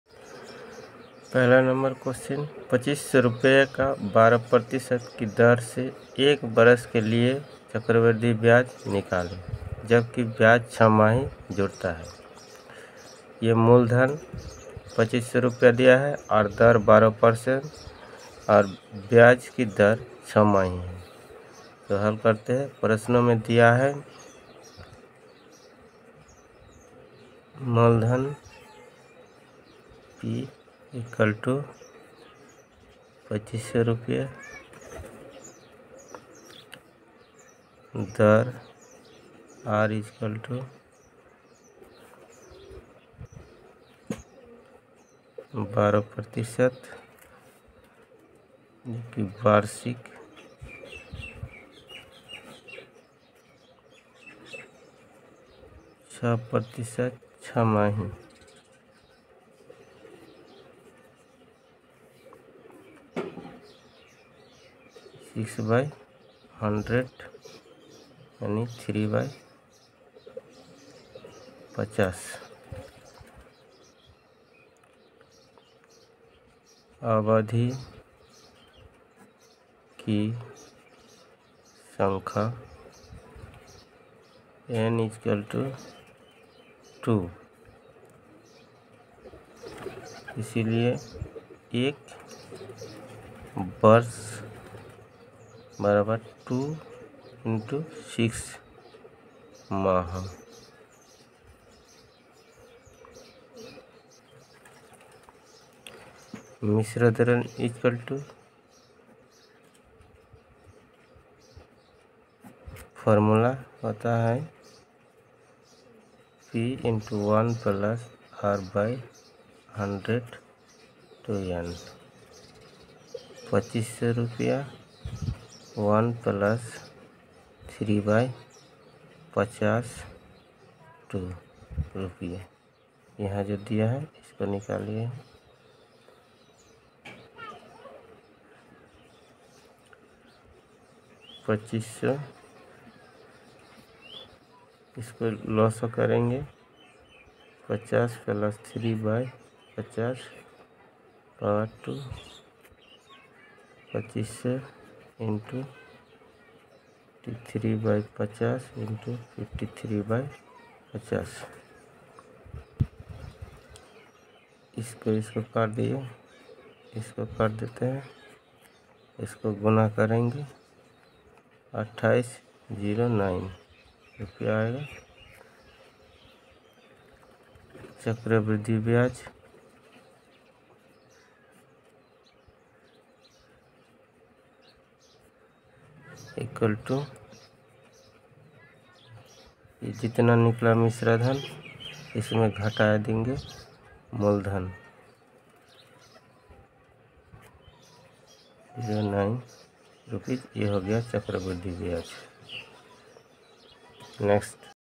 How to Find Compound interest ?//चक्रवृद्धि ब्याज कैसे निकालें//For Class X ka Top Question
How to Find the compound interest? चक्रवृद्धि ब्याज कैसे निकालें
वर्ग दस का सबसे अच्छा प्रशन
1.2500 रुपए का 12 % की दर से 1 वर्ष के लिए चक्रवृद्धि ब्याज निकाले जबकि ब्याज दर छमाही जुड़ता है?Maths compoundinterest how to find the compound interest